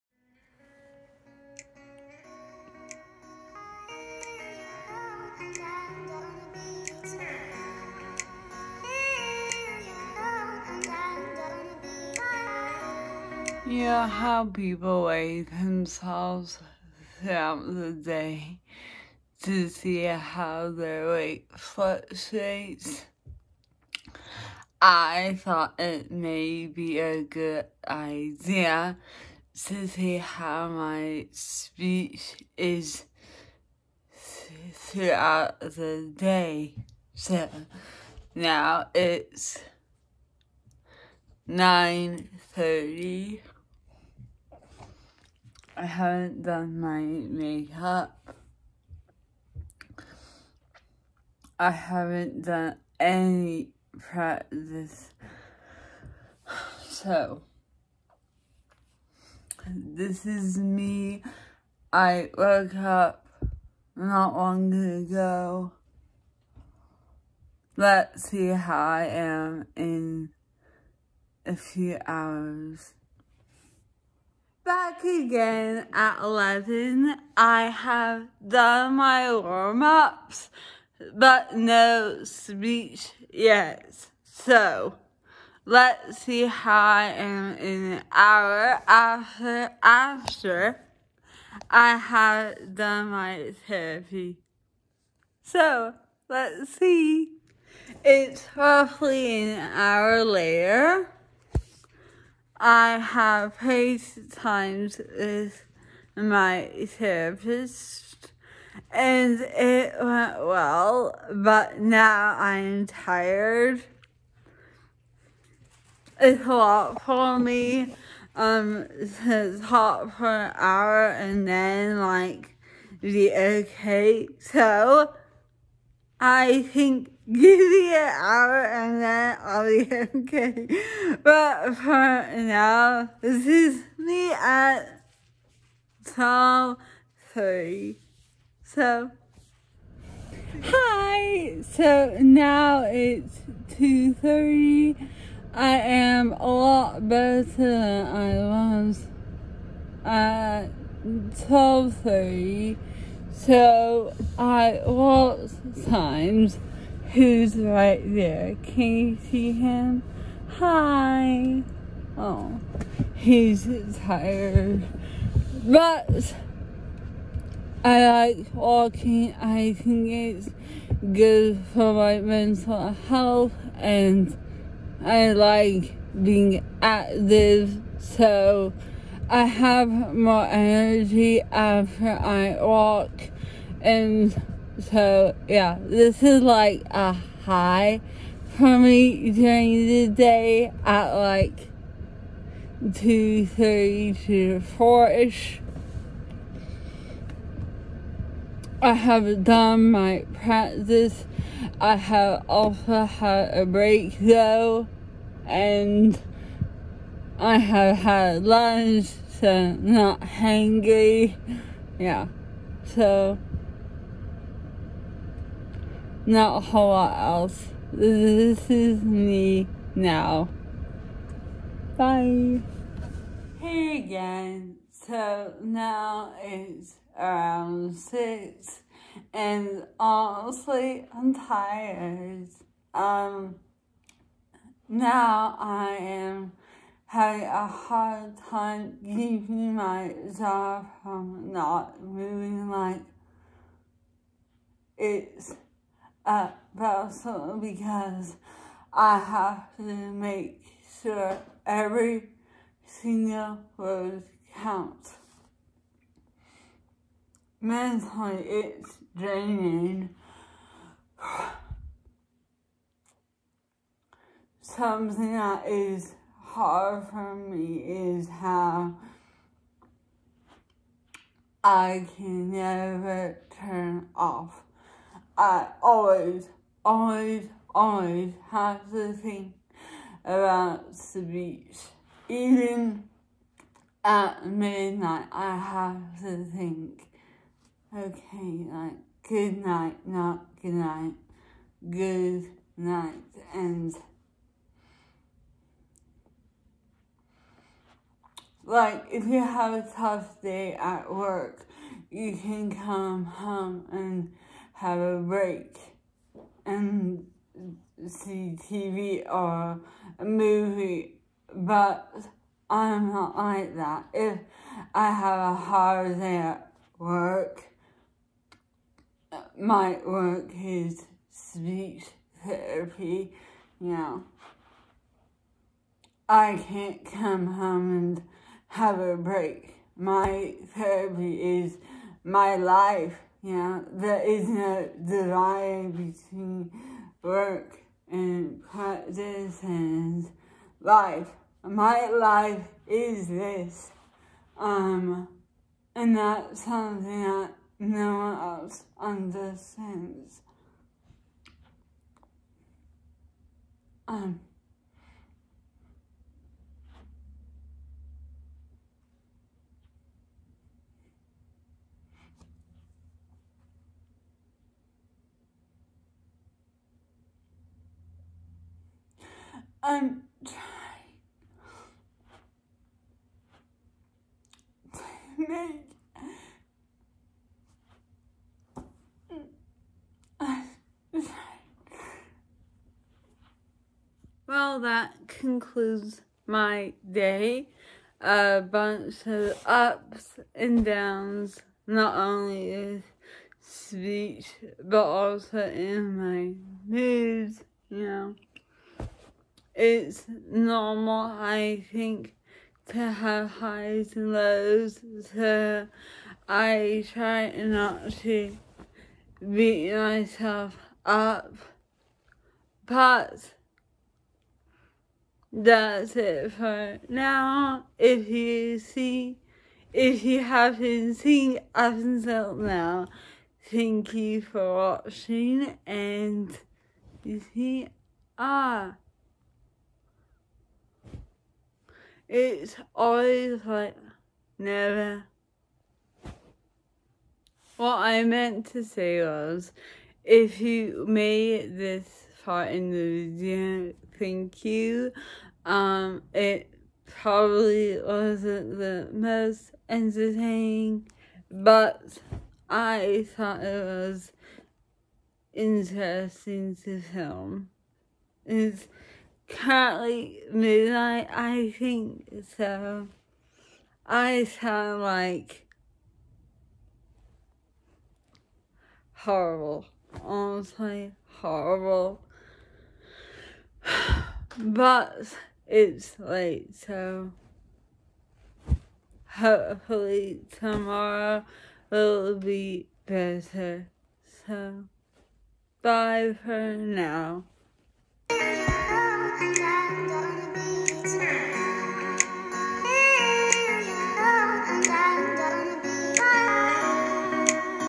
A Day in the Life with Dysarthria
I recorded myself throughout the day to document my speech changes and the effect that practice, my energy level and mood changes all effect speech. This is the most vulnerable I have ever been online. Hopefully some of you find this interesting in some way!!